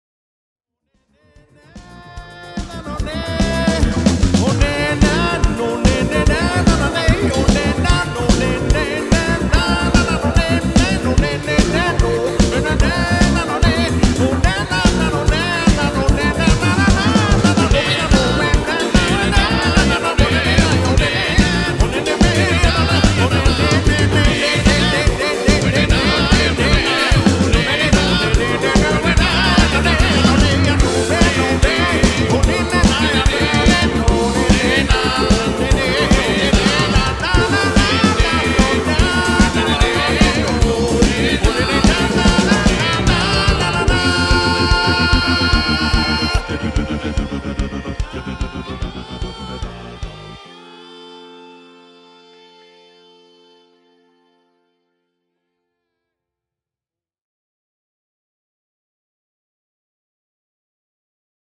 Recording Drums for a Yet Unnamed Album
Drums: Karen Teperberg
Vocals: Nir Yaniv
Other instruments: None